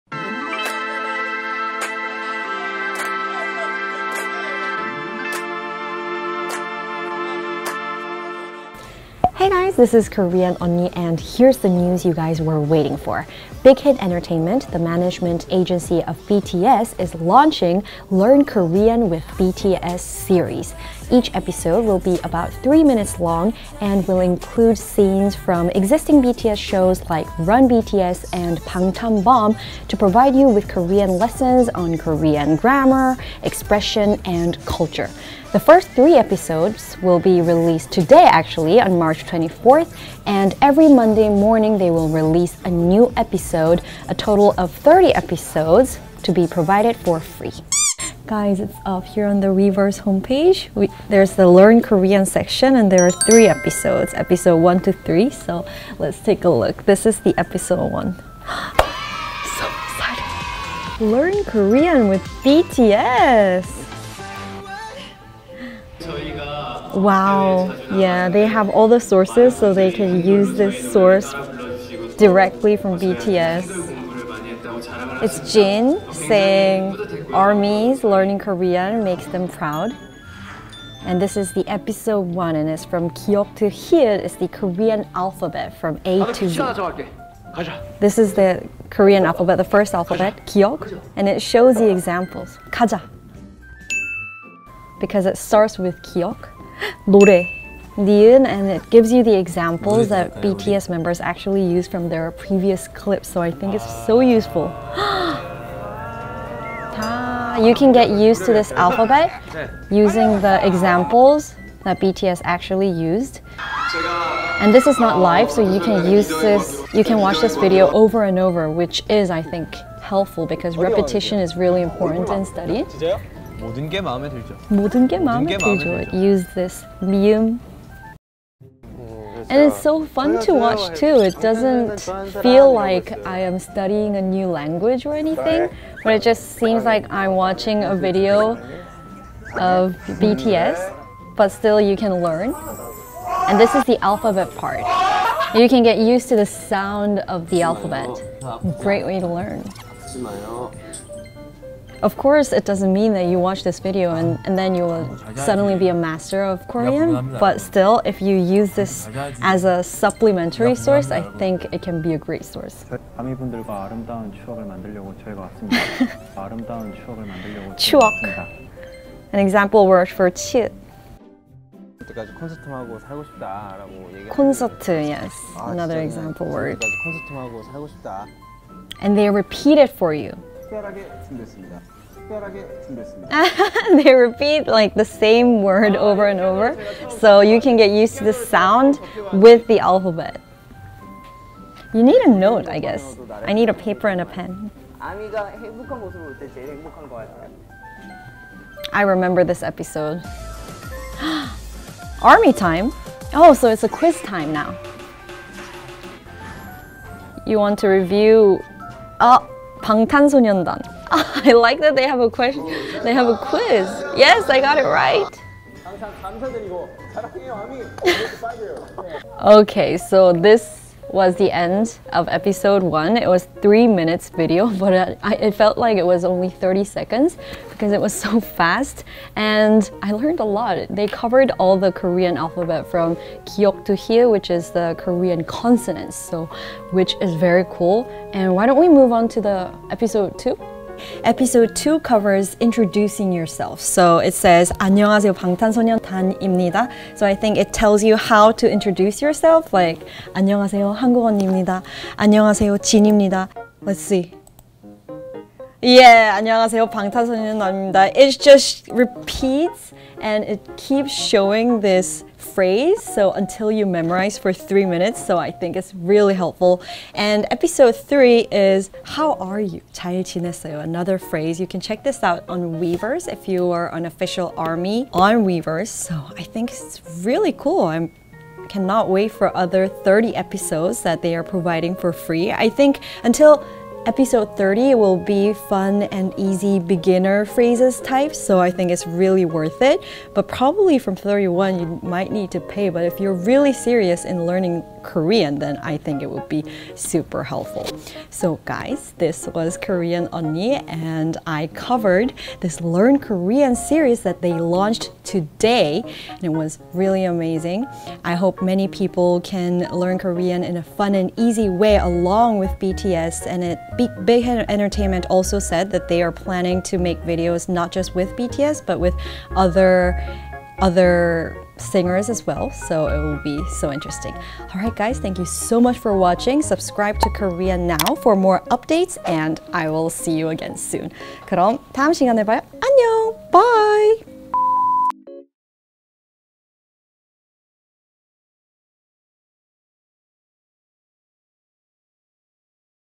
“Learn Korean with BTS” review
Big Hit Entertainment, the management agency of BTS
is launching “Learn Korean with BTS” series
Each episode will be about 3 minutes long
and will include scenes from existing BTS shows like
“Run BTS!” and “Bangtan Bomb” to provide with
Korean lessons on Korean grammar, expression, and culture
The first three episodes will be released today (Mar. 24) actually